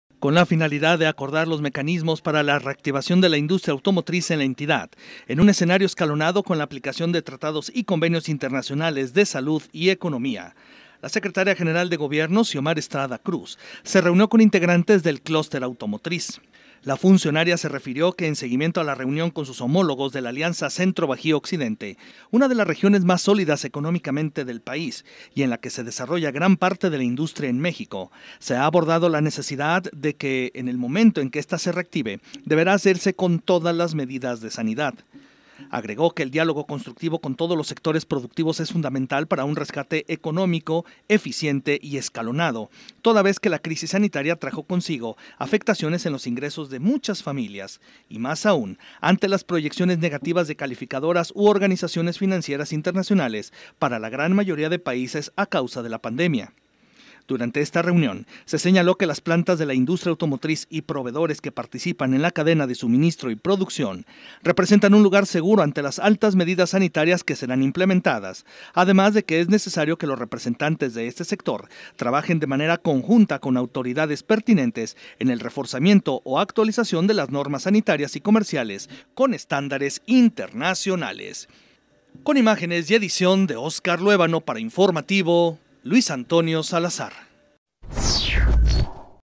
LISTOS PARA REGRESAR
Acuerdan autoridades de gobierno y del cluster automotriz, los mecanismos para regreso de esta actividad en el estado.